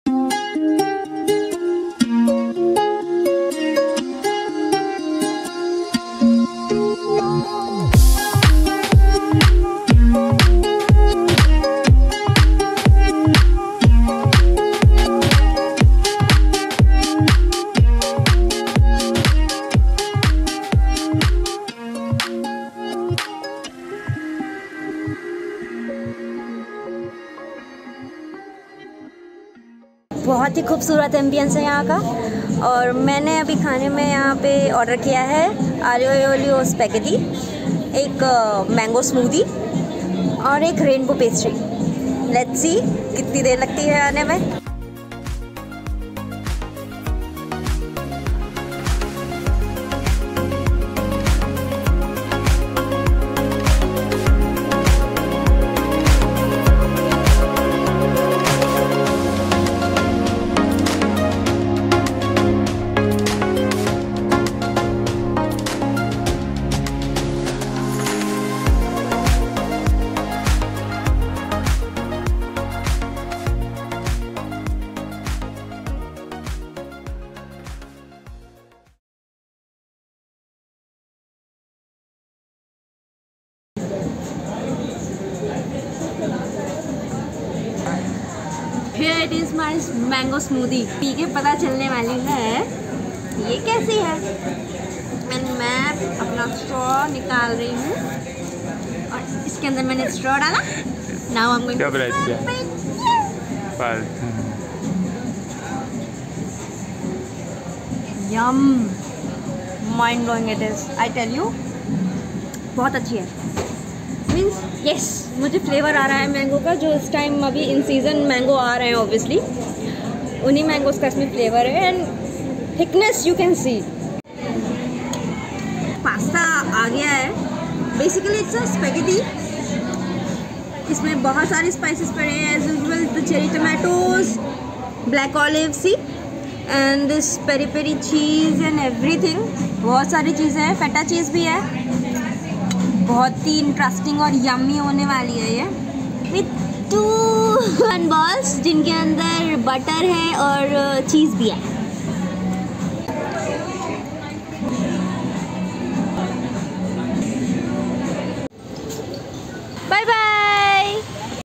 Buttercup bungalow bakery | An aesthetic place to chill out | food vlog
Apne din ko orr zada interesting banane k liye mai gyi thi Buttercup bunglow, jo ki located h Baldev Singh Arya Marg p. Baat krty h ambience ki.
Bahut he khoobsurat kisi kavi ki kalpna jaisa sundar😇. Enkijo celling thi wo peony or roses flowers se placed over thi jo ki jo kisi ka b dhyan apni orr kheechne k liye kaffi hai. Wahn p ek orange tree b tha jisme oranges k sath sath grapes b thy🤣.
Ab aaty h khane p. To sbse phly mne mngwayi rainbow pastry which was good in taste but thordi si hard lgi mere ko😯. 2nd cheez jo mne mngayi thi wo thi mango smoothie that was yummm😋😋😋😋, and last m mne mngaya tha aglio-e-olio pasta jo ki tha cheery on the top😘.